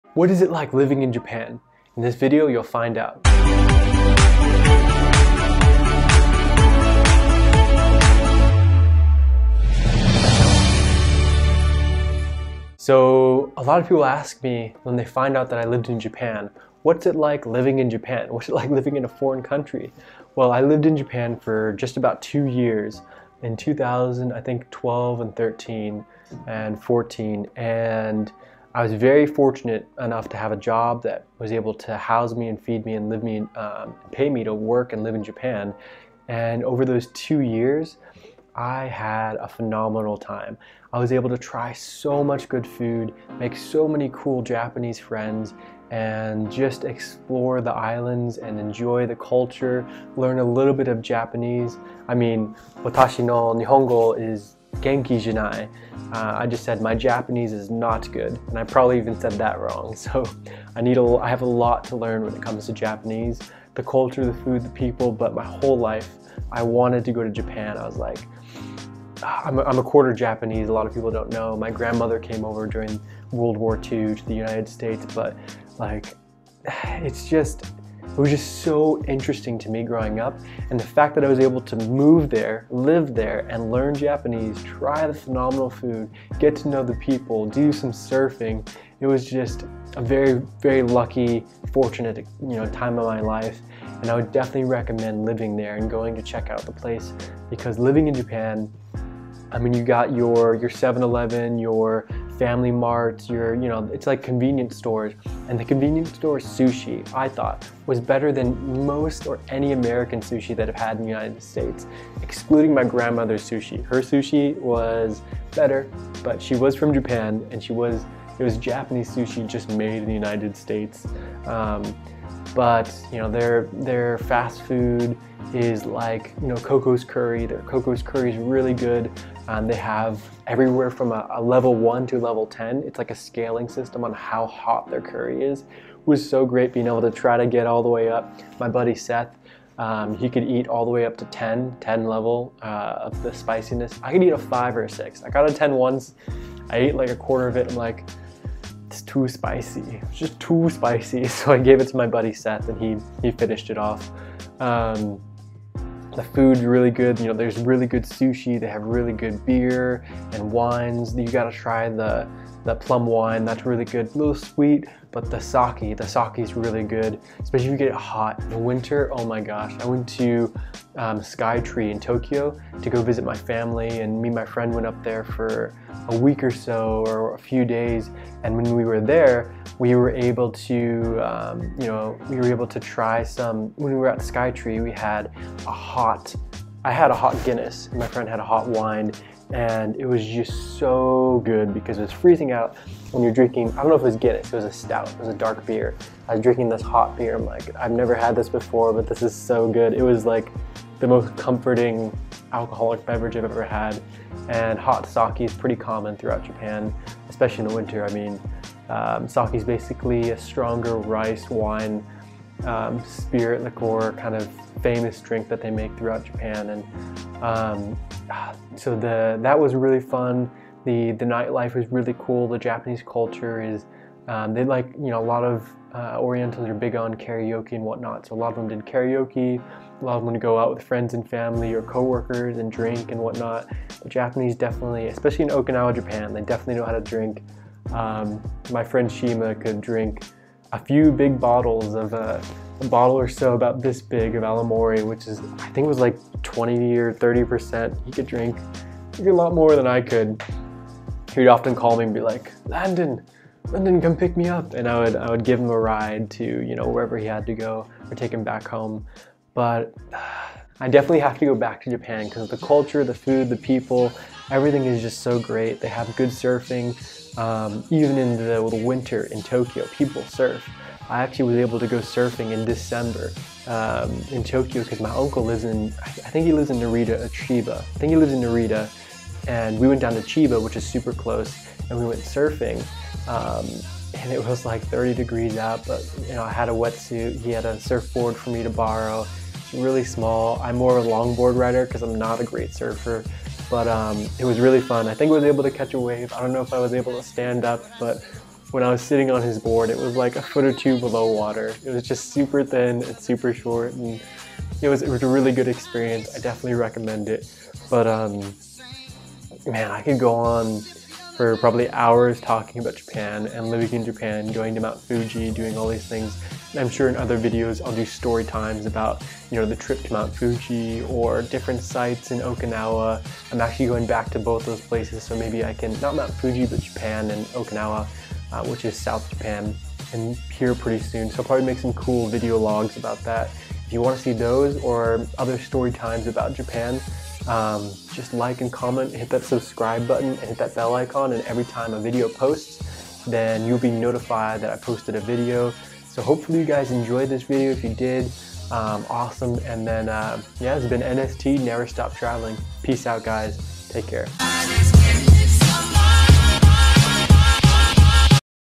#48 WHAT IS IT LIKE LIVING IN JAPAN!?!
In this video I talk about what it is like to live in japan and how amazing and different it is in compared to some laces. It is really a country that everyone should go to if the have the chance.

Pocket Juice portable charger:

The North Face Down Jacket:

Memory foam neck pillow:

Vivobarefoot Men's Tracker boots:

Columbia Silver Ridge Pants: